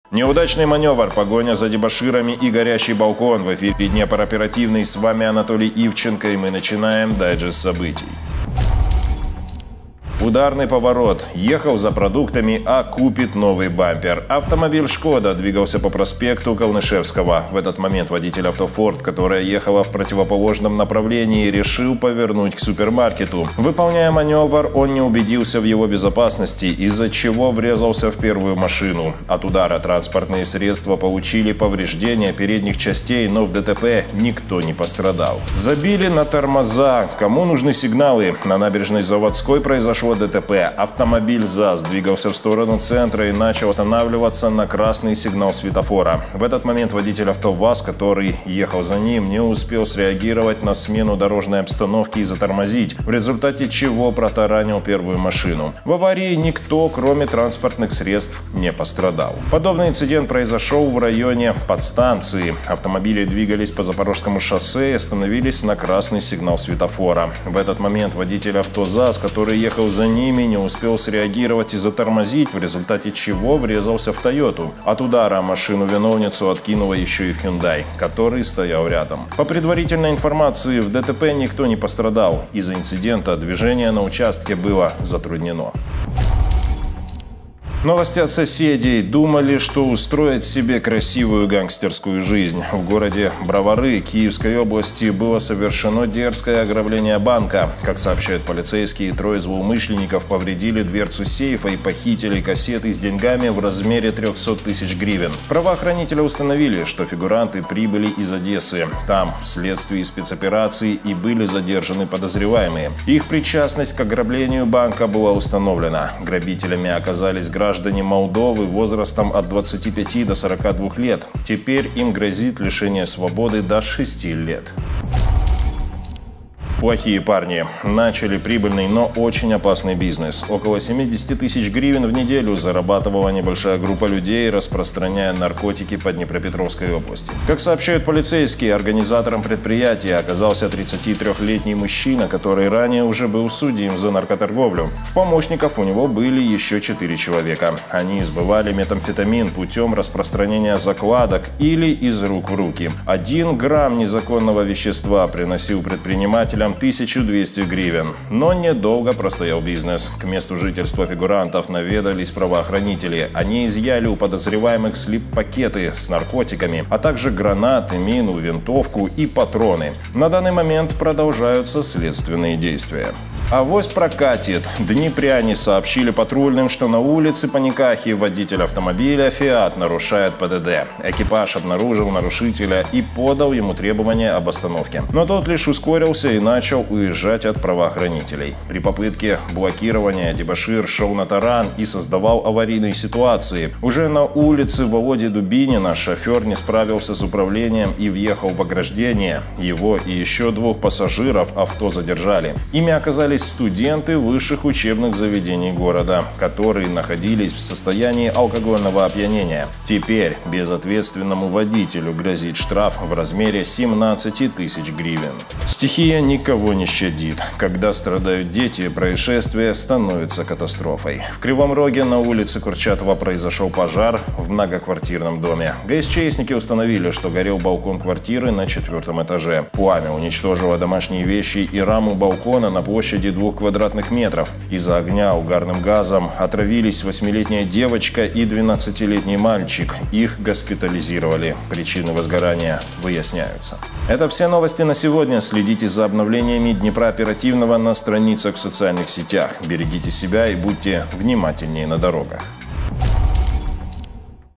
Днепр Оперативный | Происшествия, ДТП, Задержания
Неудачный манёвр, погоня за дебоширами и горящий балкон. В эфире Днепр Оперативный, с вами Анатолий Ивченко и мы начинаем дайджест событий.
Ударный поворот. Ехал за продуктами, а купит новый бампер. Автомобиль Шкода двигался по проспекту Калнышевского. В этот момент водитель авто Форд, которое ехало в противоположном направлении, решил повернуть к супермаркету. Выполняя манёвр, он не убедился в его безопасности, из-за чего врезался в первую машину. От удара транспортные средства получили повреждения передних частей, но в ДТП никто не пострадал.
Забили на тормоза Кому нужны сигналы – на Набережной Заводской произошло ДТП. Автомобиль ЗАЗ двигался в сторону центра и начал останавливаться на «красный» сигнал светофора. В этот момент водитель авто ВАЗ, который ехал за ним, не успел среагировать на смену дорожной обстановки и затормозить, в результате чего протаранил первую машину. В аварии никто, кроме транспортных средств, не пострадал.
Подобный инцидент произошёл и в районе Подстанции. Автомобили двигались по Запорожскому шоссе и остановились на «красный» сигнал светофора. В этот момент водитель авто ЗАЗ, который ехал за ними, не успел среагировать и затормозить, в результате чего врезался в Toyota. От удара машину-виновницу откинуло ещё и в Hyundai, который стоял рядом. По предварительной информации, в ДТП никто не пострадал. Из-за инцидента движение на участке было затруднено.
Новости от соседей. Думали, что устроят себе красивую гангстерскую жизнь. В городе Бровары, Киевской области, было совершено дерзкое ограбление банка. Как сообщают полицейские – трое злоумышленников повредили дверцу сейфа и похитили кассеты с деньгами в размере 300 тысяч гривен. Правоохранители установили, что фигуранты прибыли из Одессы. Там, в следствии спецоперации, и были задержаны подозреваемые. Их причастность к ограблению банка была установлена. Грабителями оказались граждане Молдовы возрастом от 25 до 42 лет. Теперь им грозит лишение свободы до 6 лет.
Плохие парни. Начали прибыльный, но очень опасный бизнес. Около 70 тысяч гривен в неделю зарабатывала небольшая группа людей, распространяя наркотики по Днепропетровской области. Как сообщают полицейские, организатором предприятия оказался 33-летний мужчина, который ранее уже был судим за наркоторговлю. В помощниках у него были ещё четыре человека. Они сбывали метамфетамин путём распространения закладок или «из рук в руки». Один грамм незаконного вещества приносил «предпринимателям» 1200 гривен. Но недолго простоял бизнес. К месту жительства фигурантов наведались правоохранители. Они изъяли у подозреваемых слип-пакеты с наркотиками, а также гранаты, мину, винтовку и патроны. На данный момент продолжаются следственные действия.
Авось прокатит! Днепряне сообщили патрульным, что на улице Паникахи водитель автомобиля Фиат нарушает ПДД. Экипаж обнаружил нарушителя и подал ему требование об остановке. Но тот лишь ускорился и начал уезжать от правоохранителей. При попытке блокирования дебошир шёл на таран и создавал аварийные ситуации. Уже на улице Володи Дубинина шофёр не справился с управлением и въехал в ограждение. Его и ещё двух пассажиров авто задержали. Ими оказались студенты высших учебных заведений города, которые находились в состоянии алкогольного опьянения. Теперь безответственному водителю грозит штраф в размере 17 000 гривен.
Стихия никого не щадит. Когда страдают дети, происшествие становится катастрофой. В Кривом Роге на улице Курчатова произошёл пожар в многоквартирном доме. ГСЧСники установили, что горел балкон квартиры на четвёртом этаже. Пламя уничтожило домашние вещи и раму балкона на площади 2 квадратных метра. Из-за огня угарным газом отравились 8-летняя девочка и 12-летний мальчик, их госпитализировали. Причины возгорания выясняются.
Это все новости на сегодня. Берегите себя и будьте внимательнее на дорогах.

Днепр Оперативный - Самые свежие новости про ДТП/ЧП и криминальную хронику в Днепре.